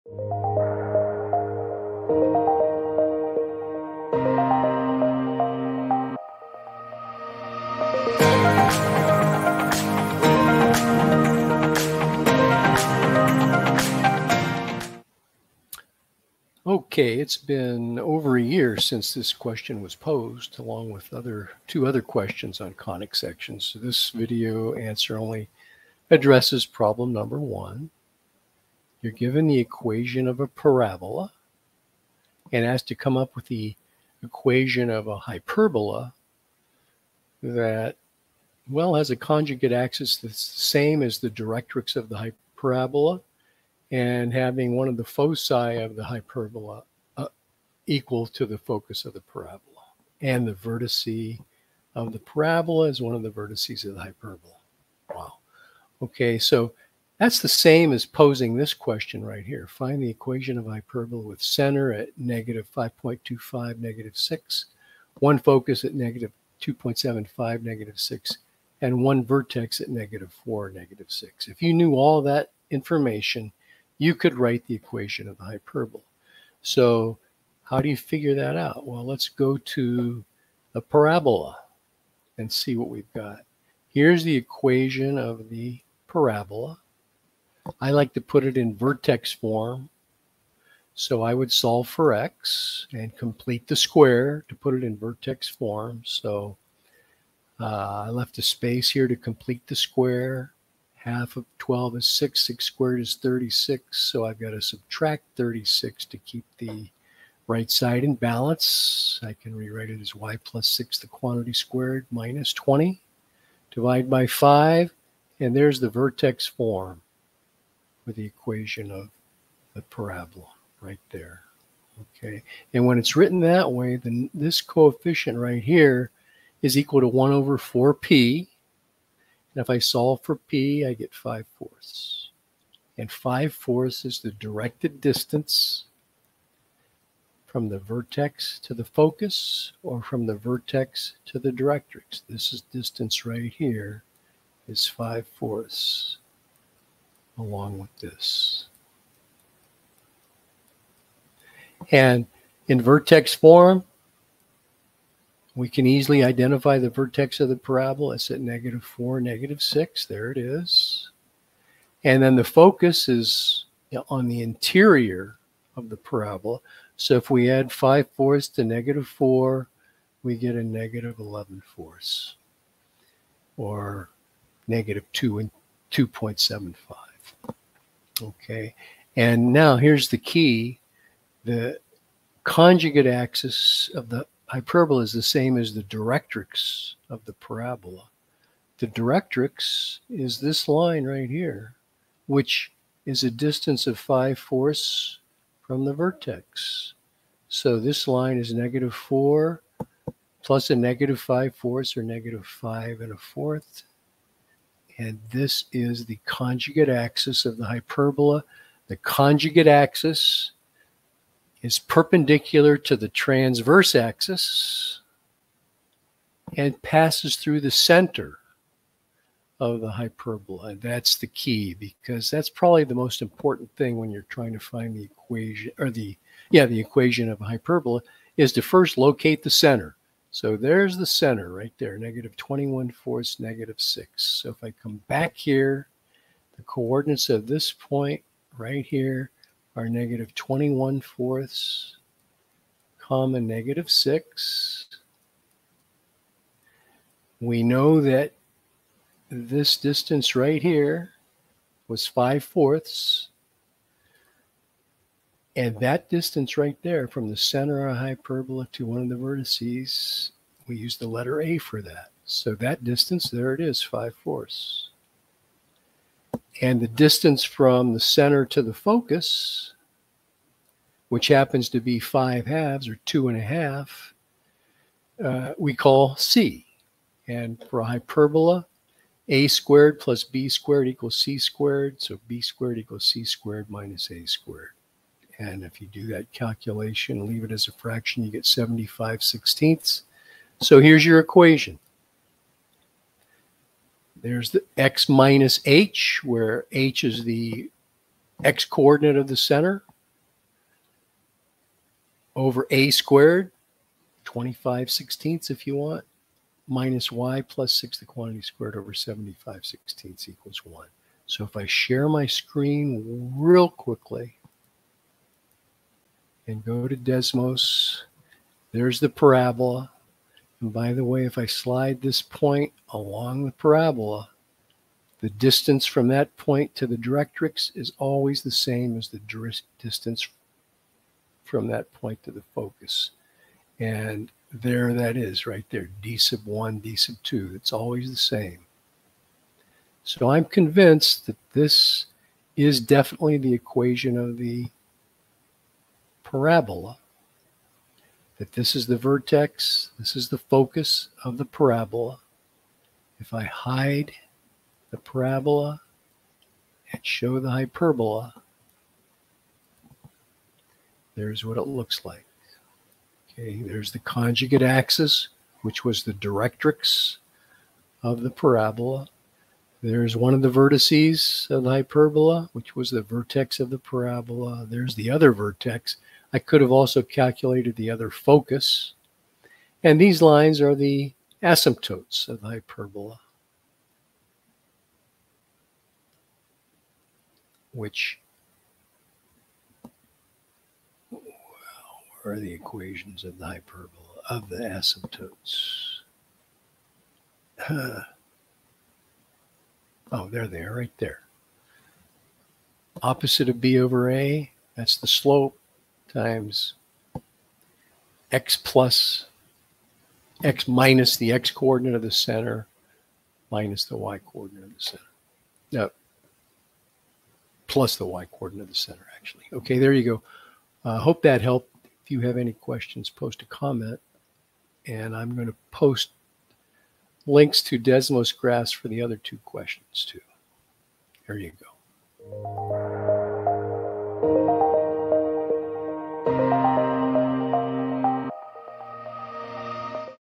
Given the equation of a parabola find equation of hyperbola sharing conjugate axis with directrix +
Question: Problems Involving Conic Sections
 1.   Find the standard equation of the hyperbola whose conjugate axis is on the directrix of the parabola y2 -5x+12y=-16, having the focus of the parabola as one of its foci, and the vertex of the parabola as one of its vertices. 2.   Find the standard equation of the parabola opening to the left whose axis contains the major axis of the ellipse  4x2 +9y2 =144, whose focus is the center of the ellipse, and which passes through the co-vertices of this ellipse. 3.   Find the standard equation of the ellipse whose major and minor axes are the transverse and conjugate axes (not necessarily in that order) of the hyperbola y2 -x2-4x-4y-4=0

Answered By:

Doug C.

Written Explanation:

is a Desmos graph for the parabola in part 2 for part 3